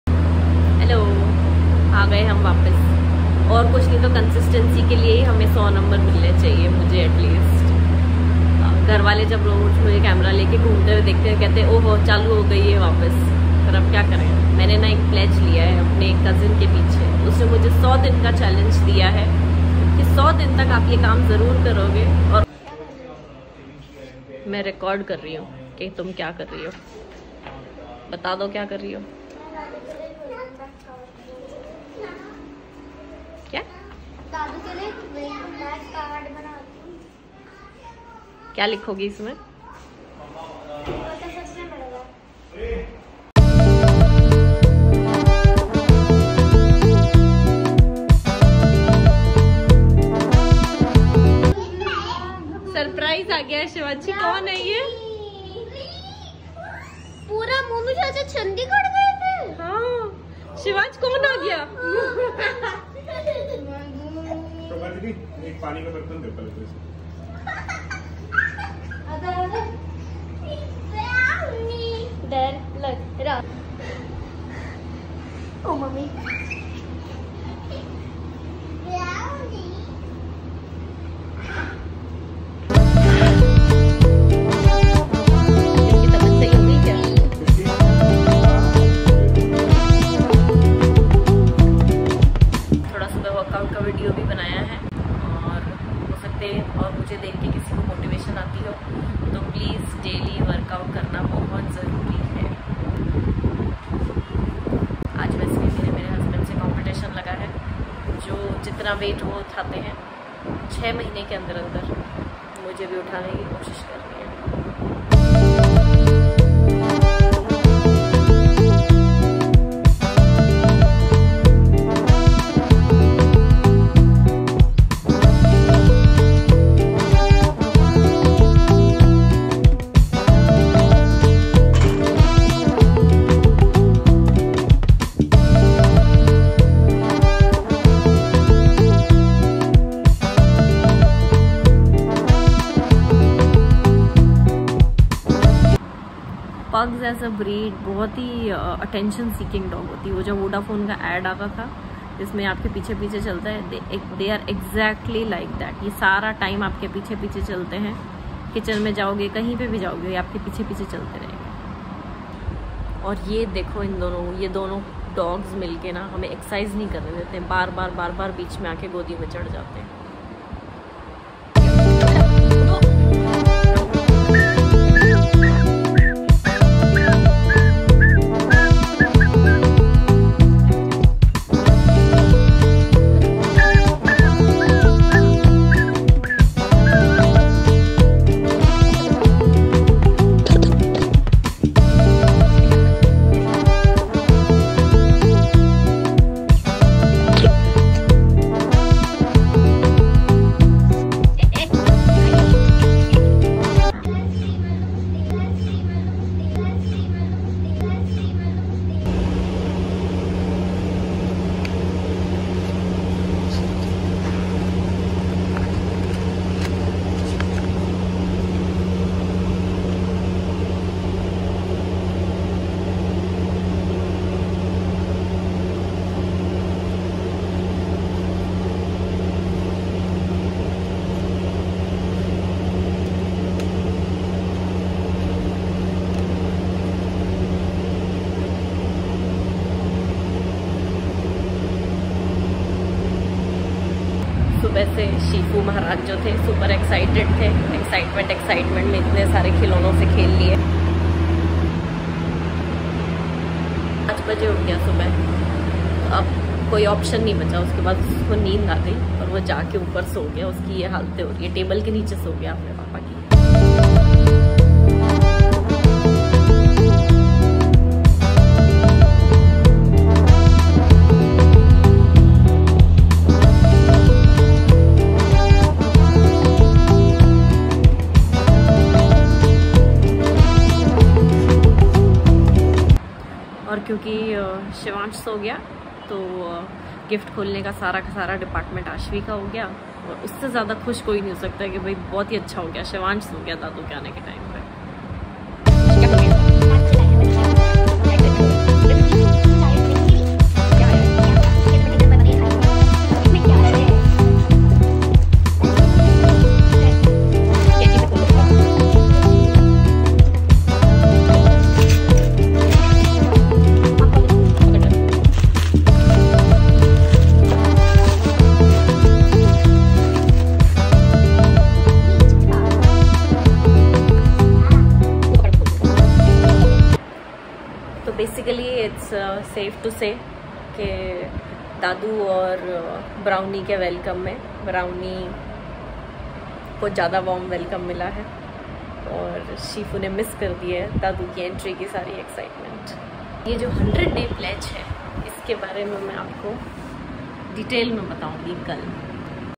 Shifu ne dadu ki entry miss kar di!!
Naye dog ke ghar me aane se itne excited ho gaye bache ki usko aapa pass nachte rahe sara din.we have a new Vodafone dog at home now, one is 13 year old and one is 3 month old.
In sare excitement me shifu was tired and slept off and aashu got all the sweets that dadu got from turkey. Turkish Baklava humne pehli baar khaya.
Also is video me hum deadlift kar rahe hai mai 40 kilo se aur pati dev 60 kilo se